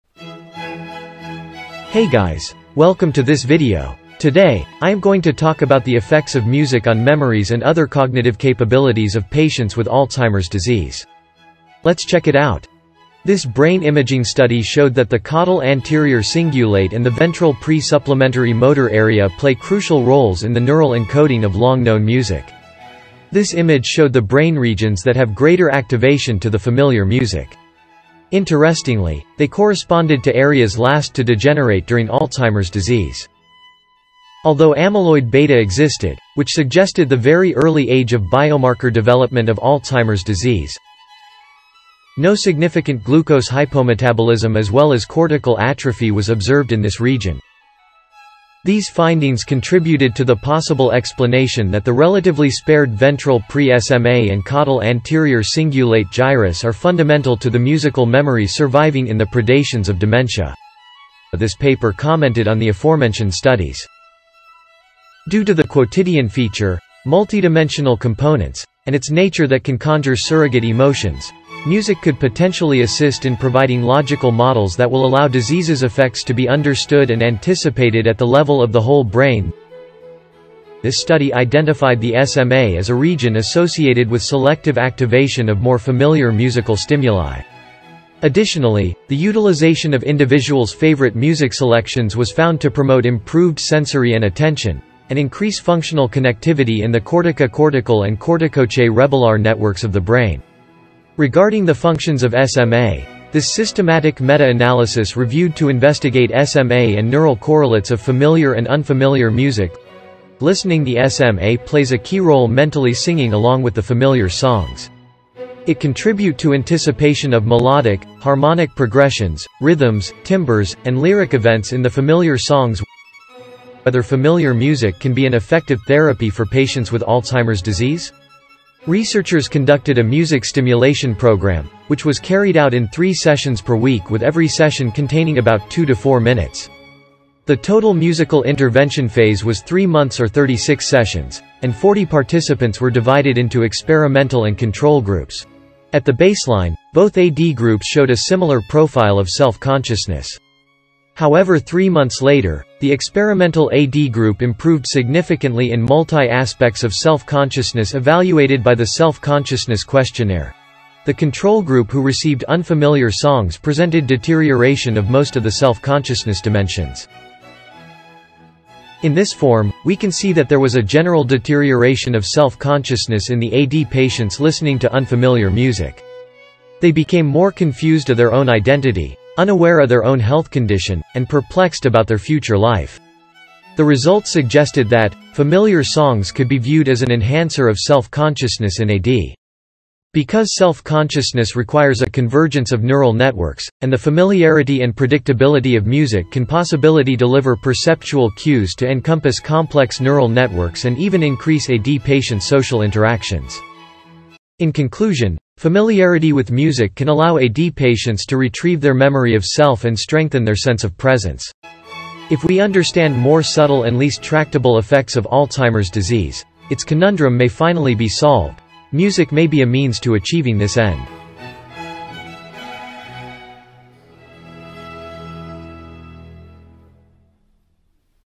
Neural Mechanisms Underlying Music Therapy for Alzheimer’s Disease
Music & Brain Science

References:

Arroyo-Anllo, E.M., Diaz, J.P. & Gil, R. (2013) Familiar Music as an Enhancer of Self-Consciousness in Patients with Alzheimer's Disease. Biomed Res Int, 2013.
Clark, C.N. & Warren, J.D. (2015) Music, memory and mechanisms in Alzheimer's disease. Brain, 138, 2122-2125.
Freitas, C., Manzato, E., Burini, A., Taylor, M.J., Lerch, J.P. & Anagnostou, E. (2018) Neural Correlates of Familiarity in Music Listening: A Systematic Review and a Neuroimaging Meta-Analysis. Front Neurosci, 12, 686.
Jacobsen, J.H., Stelzer, J., Fritz, T.H., Chetelat, G., La Joie, R. & Turner, R. (2015) Why musical memory can be preserved in advanced Alzheimer's disease. Brain, 138, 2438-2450.